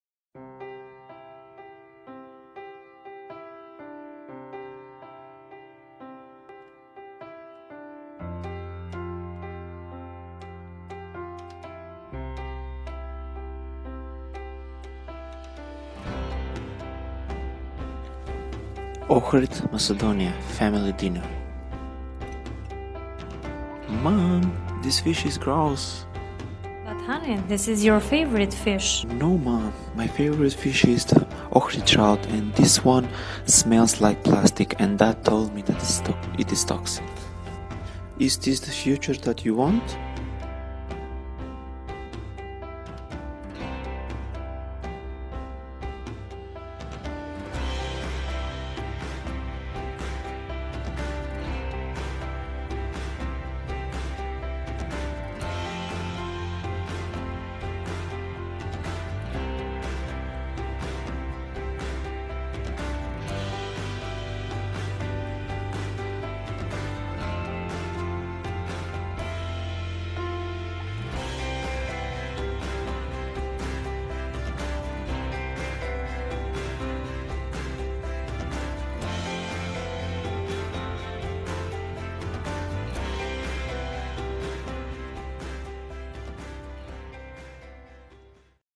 Cross Border Cooperation between Macedonia and Albania for sustainable use of Ohrid trout
Directed by: Ljupcho Vasilev
Category: Act naturally
"Treats and causes that decrease the population of Ohrid trout. Awareness for all relevant audience, who can affect to protect the Biodiversity, with aim of conservation and sustainable use  of Biodiversity of Lake Ohrid. Awareness for increasing the cross border cooperation between Macedonia and Albania."